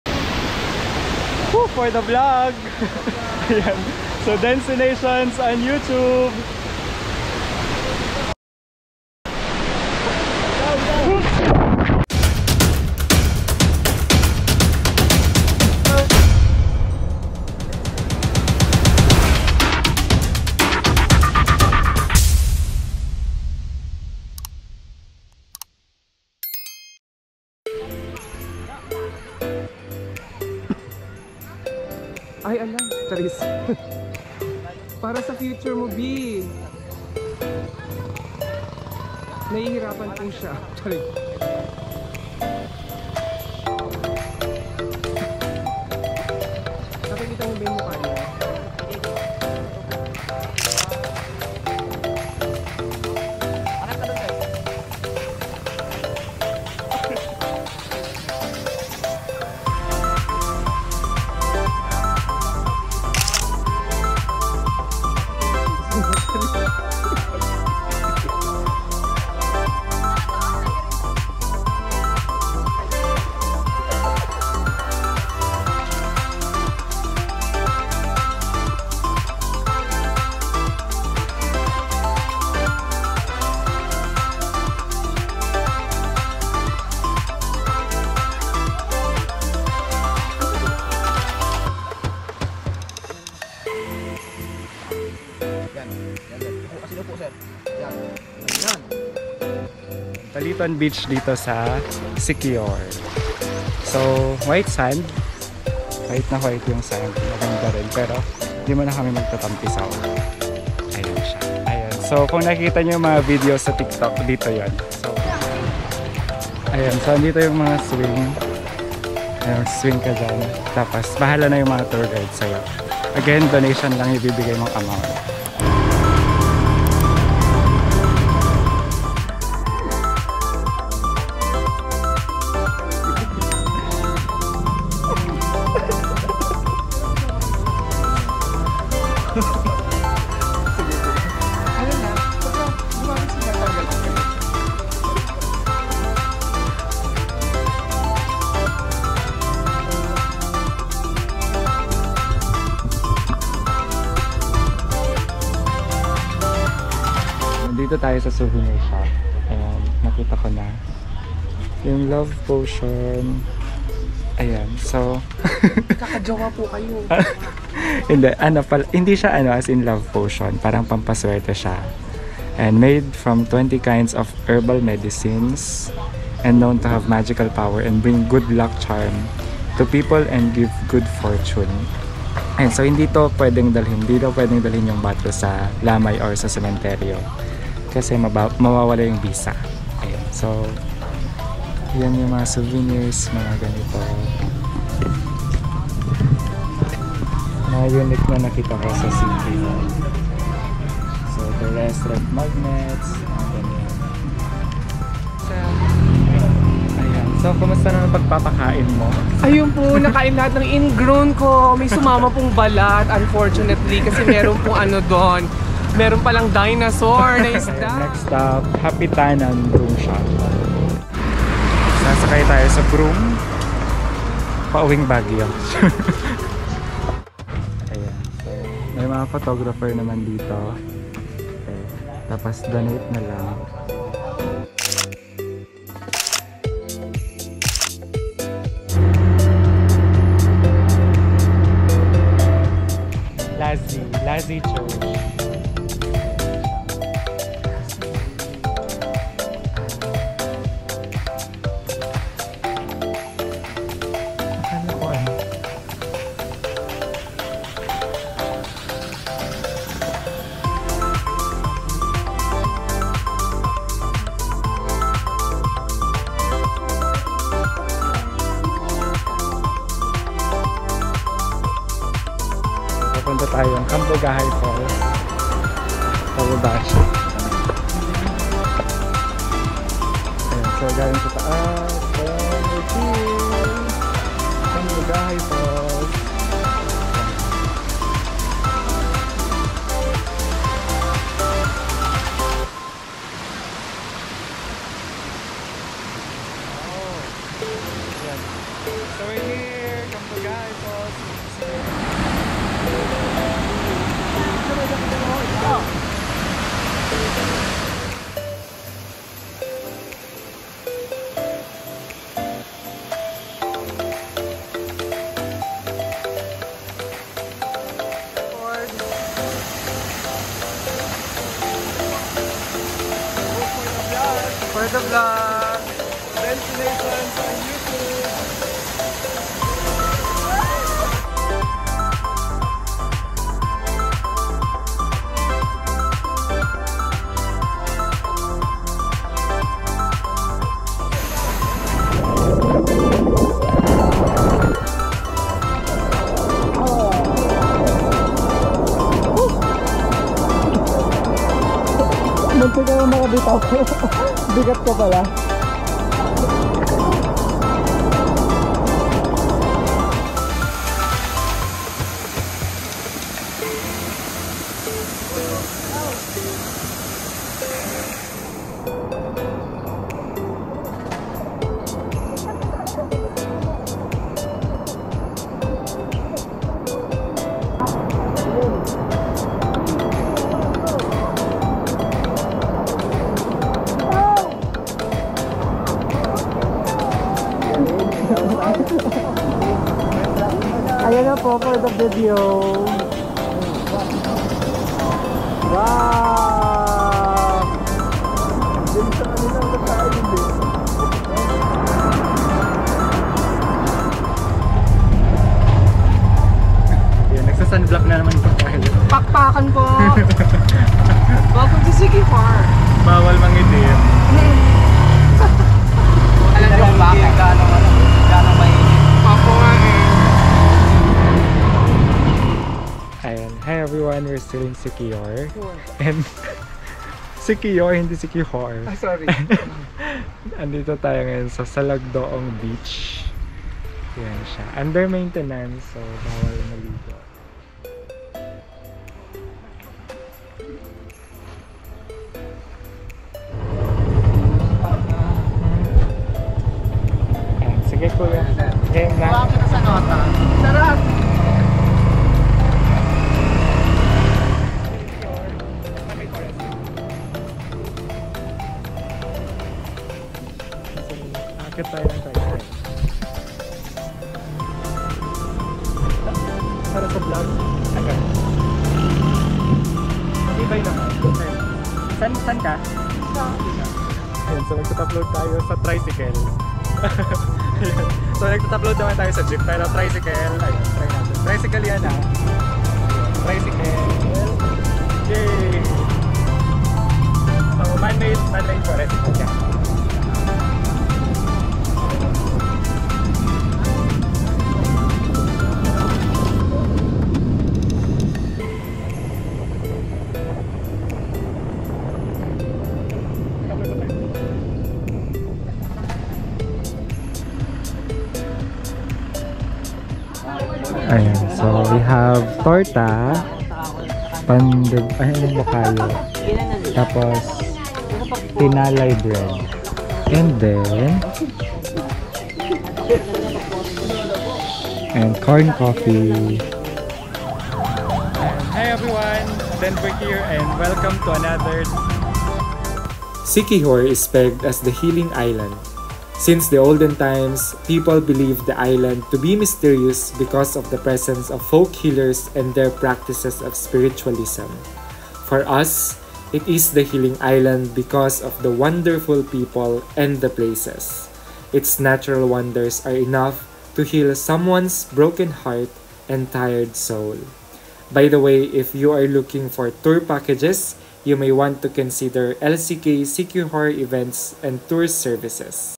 Siquijor, The Healing Island 2022 | Love Potion? | Complete Coastal Tour Package Itinerary + Budget
Welcome to another DENstination - Denver’s Travel Diaries! Join me as we explore beautiful places in the Philippines!

We are on the mystical island of Siquijor. Siquijor is pegged as 'the healing island'. Since the olden times, people believe the island to be mysterious because of the presence of folk healers and their practices of spiritualism.

For us, it is a healing island because of the wonderful people and the places. Its natural wonders are enough to heal someone's broken heart and tired soul.

If you are looking for a tour package, you may want to consider LCK Siquijor Events and Tours Services. Here is the link to their FB page:

Chapters:

00:00 Introduction
00:27 Paliton Beach
02:09 Century Old Balete Tree
02:37 Souvenir Shop (Love Potion)
03:40 Hapitanan Broom Shot
04:04 Lazi Church and Convent of Lazi
04:35 Cambugahay Falls
07:03 Salagdoong Beach
07:25 Man-made Forest
08:30 Pan Bisaya
08:55 Closing